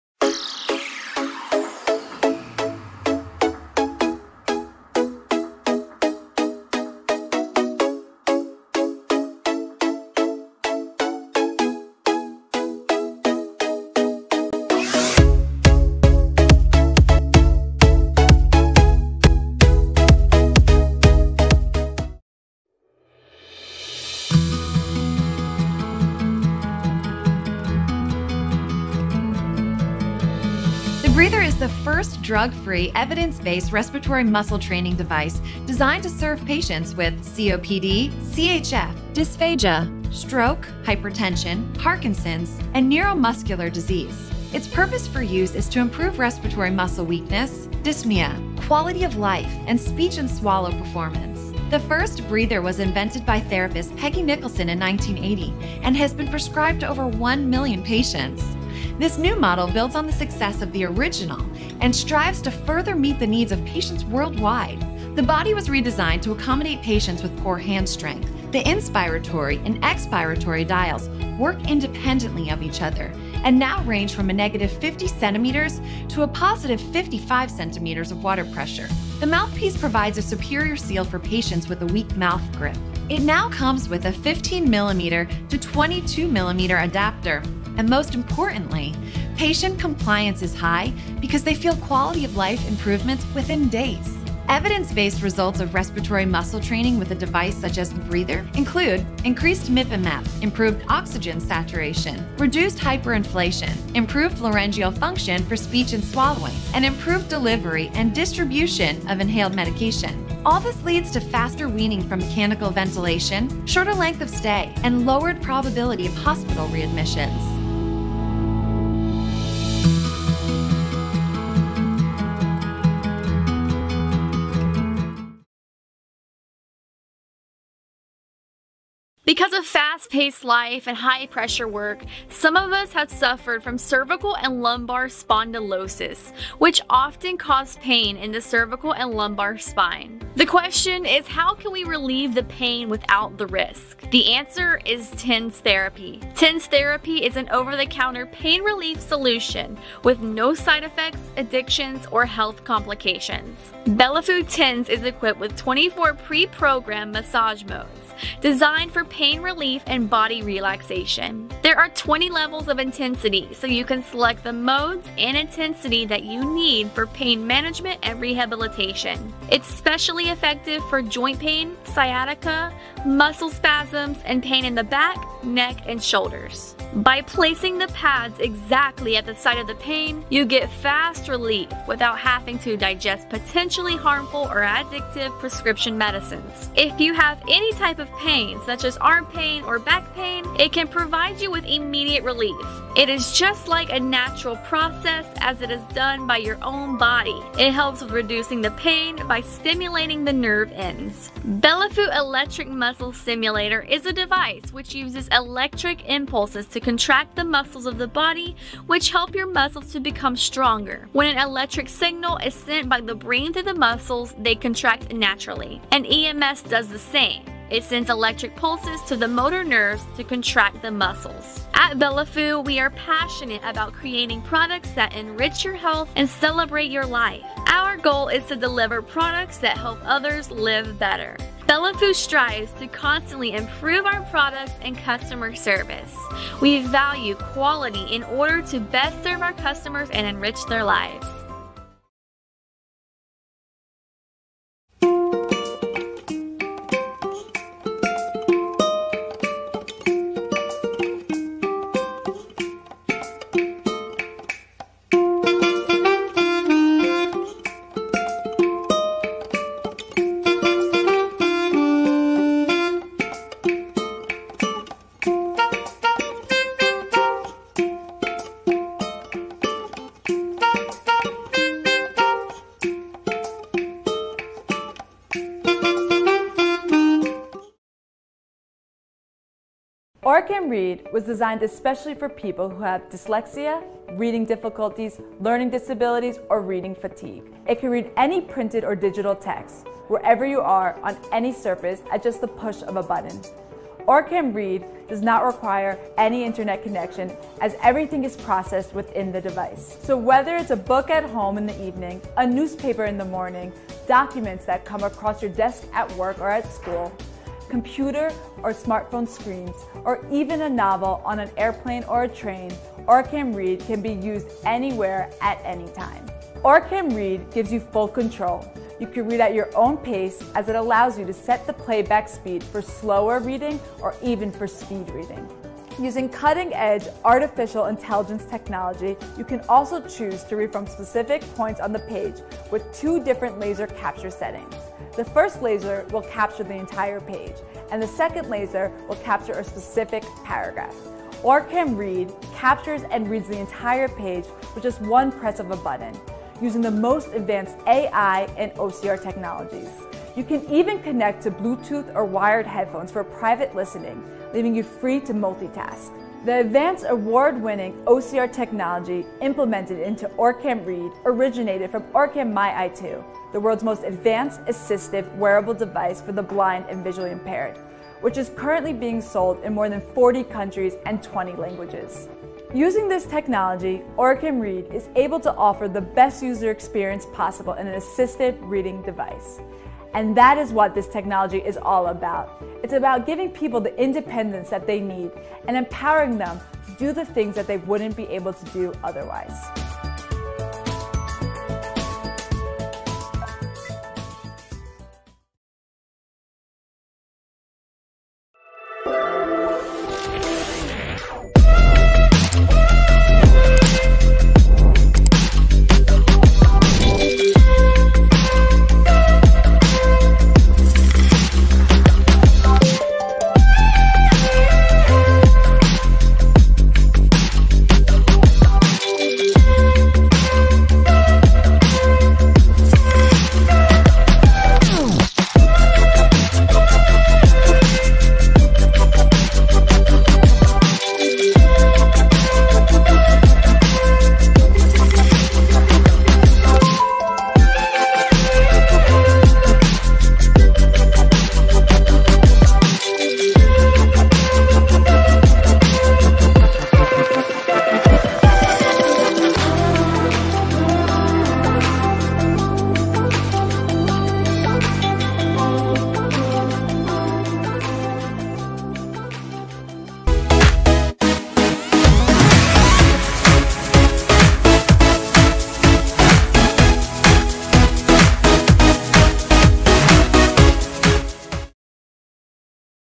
Next-Level Wellness: 5 Health Gadgets You Need to Try
Timestamps:
0:00 Intro

0:20 1. The Breather Natural Breathing Lung Recovery

2:05 2. Belifu Dual Channel TENS EMS Unit

4:02 3. Electric Feet Callus Removers

4:38 4. OrCam Read - First-of-its-Kind Artificial Intelligence (AI) Assistive Reader

6:35 5. Tranqwil Foot Massager Machine